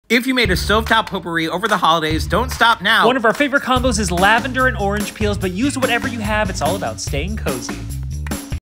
Winter Stovetop Potpourri
Stovetop potpourri is an easy eco friendly way to freshen up your home. Use your scraps and simmer them with a bit of water. If you don't want to leave it on the stove, use a crockpot! One of our favorite combinations this time of year is orange peels and lavender dar essential oils.